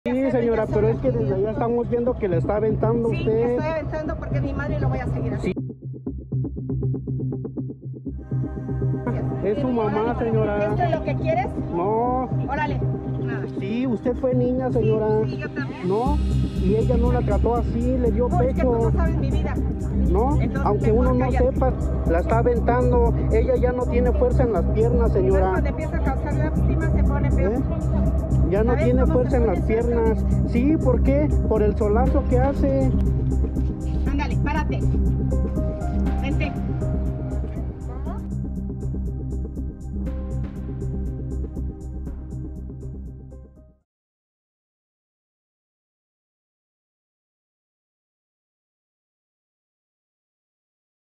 Una mujer empuja y deja en el piso a su propia madre en vía pública - N+
Una grabación se hizo viral, por evidenciar a una mujer de la tercera edad sin fuerza para ponerse en pie, luego que su hija la empujara y la dejara en el piso.